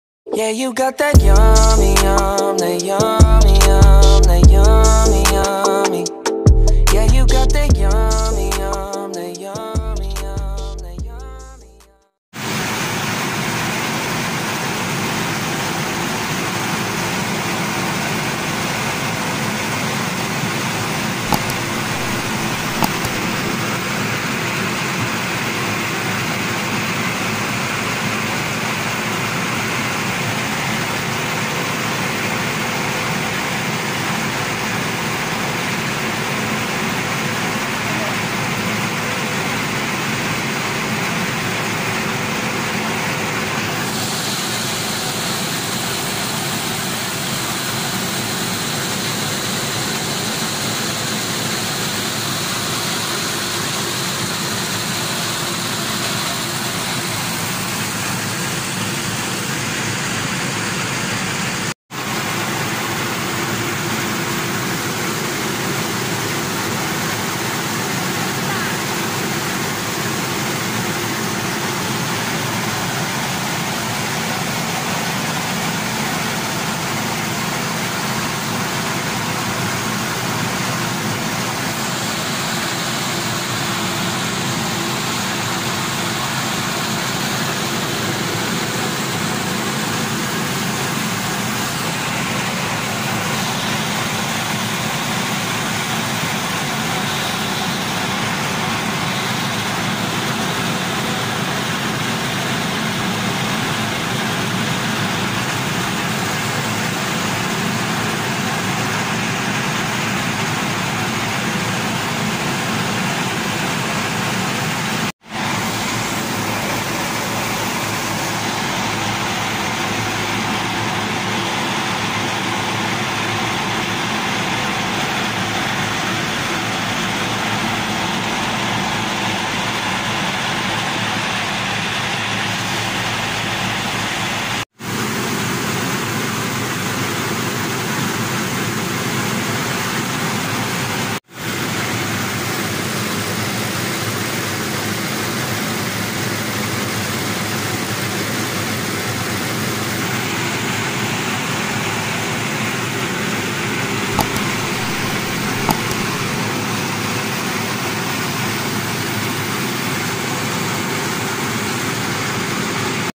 Kiskisan ng palay: ka yummy tv
Panuorin kung paano gawing bigas ang palay.
Ganito kami sa Infanta Quezon.
Suportahan natin si ka yummy.
Neil thor Evardome Tv. Salamat